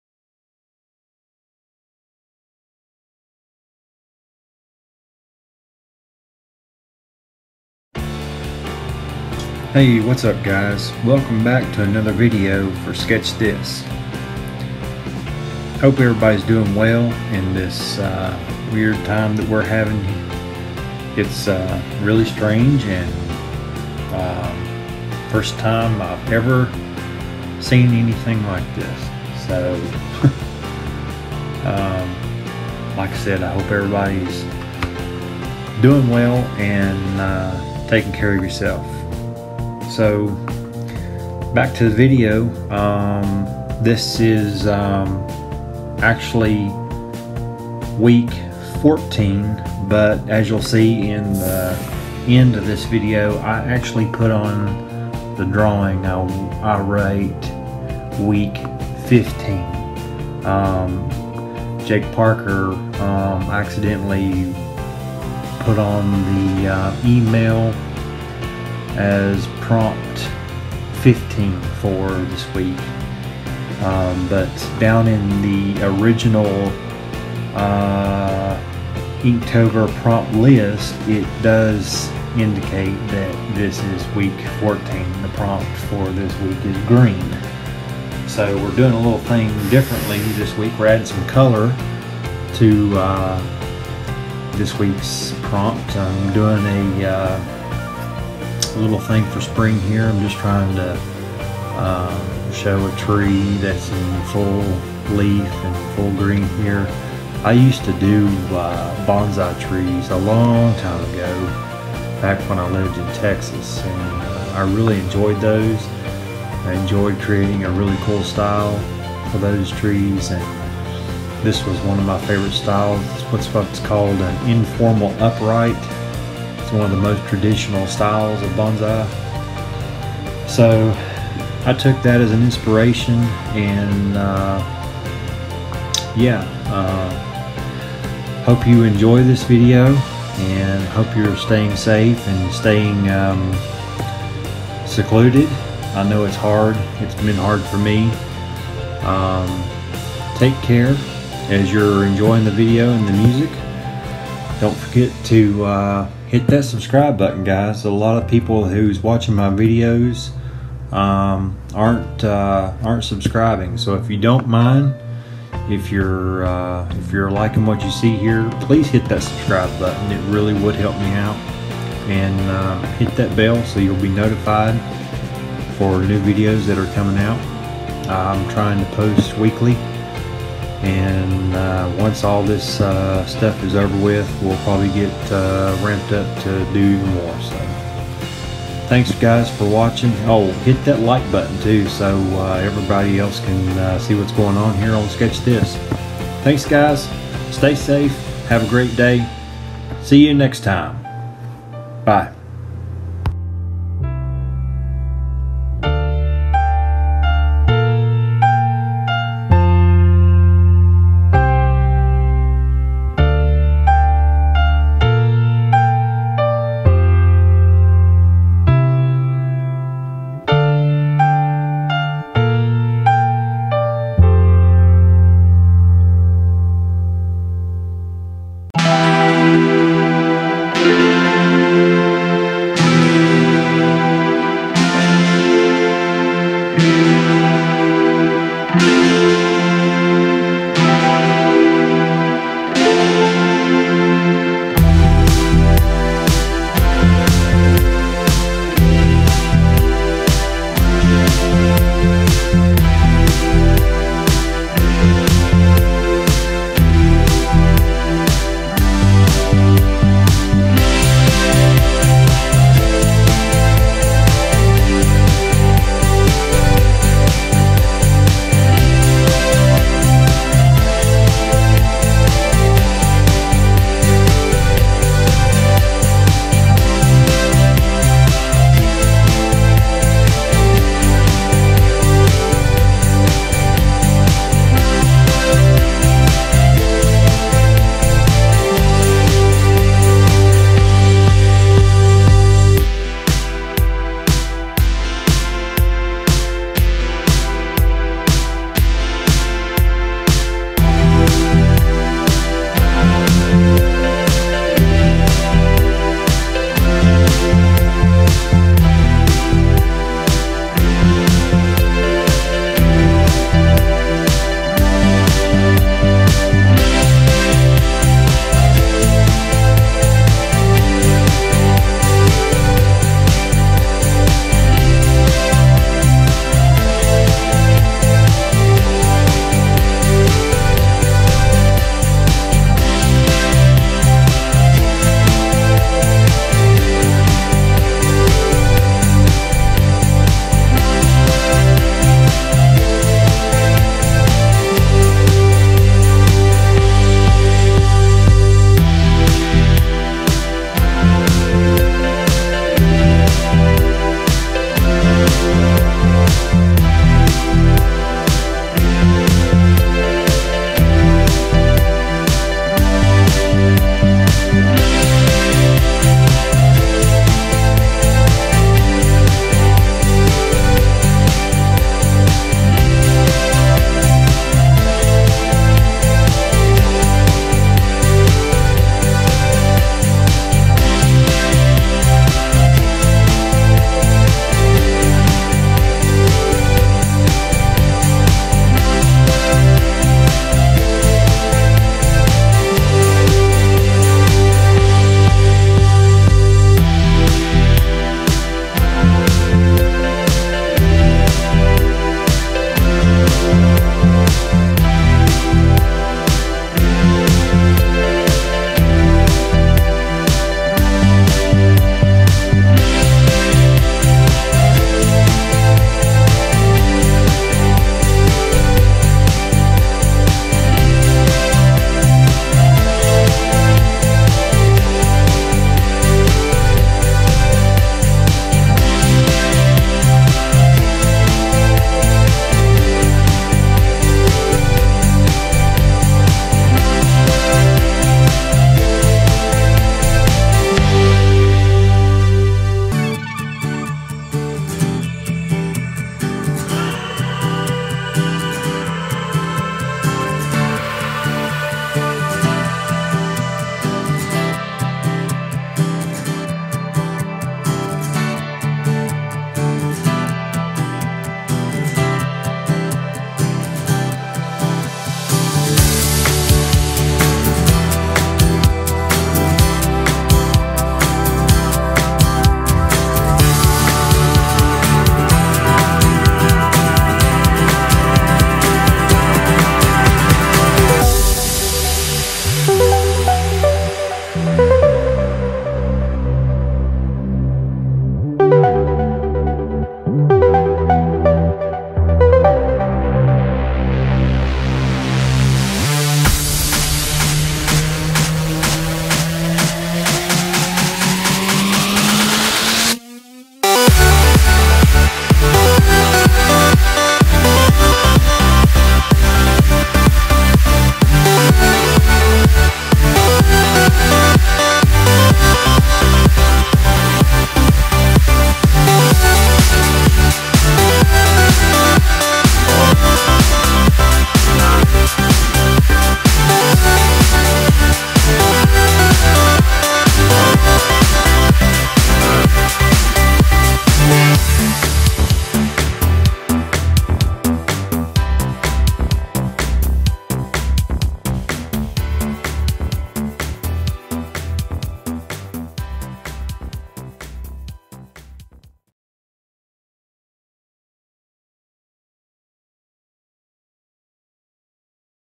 This time we add some color to inktober52 - green.
Spring is in the air and I'm so ready for it. I sketch and color one of my favorite artforms, bonsai!! For this week's prompt word green I sketch a beautiful informal upright bonsai tree in color using mastermarkers.

Supplies used:
Bristol Strathmore -  Colored Pencil 6 x 8
Micron pens: .03 - .01